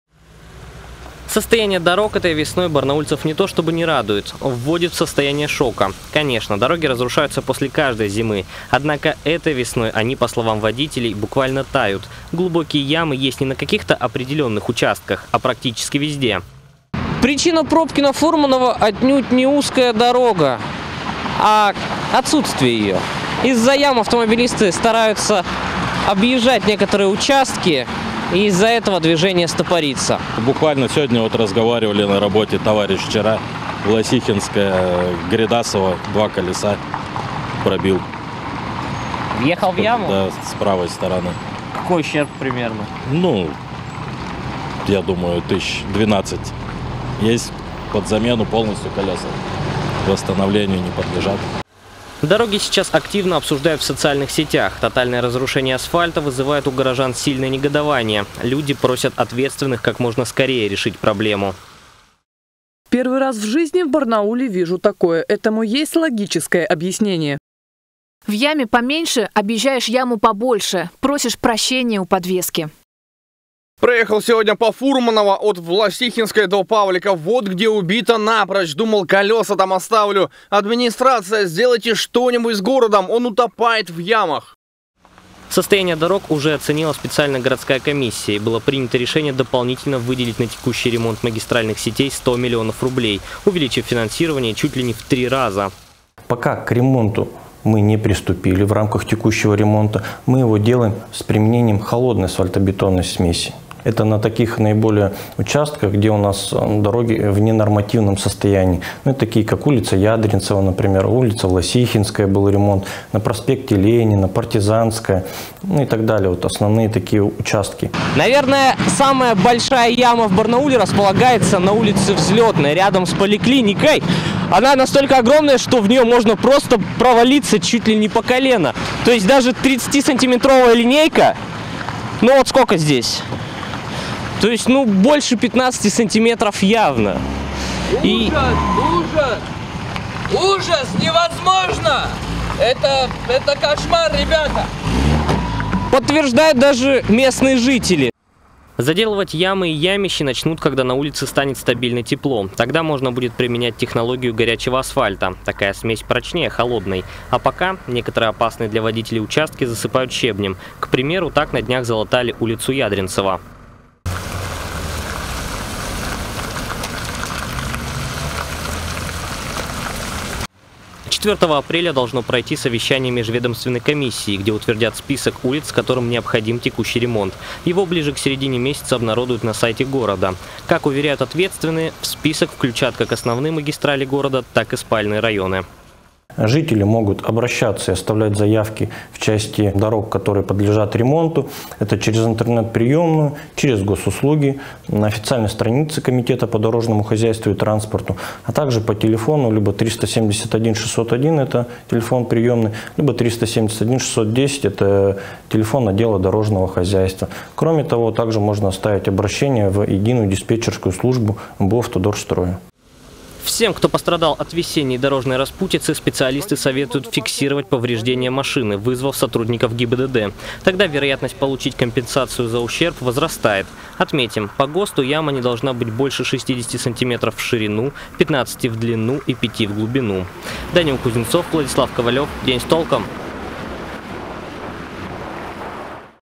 Водители возмущены: когда в Барнауле начнут ремонтировать дороги
Ежегодное весеннее обследование дорог завершилось в Барнауле. Комиссия оценила их состояние, и составила список объектов, которые ждет текущий ремонт. Автомобилисты отмечают, что после зимы многие дороги пришли в полную негодность. Когда начнут заделывать ямы? И куда обращаться, если нужно залатать  определенный участок?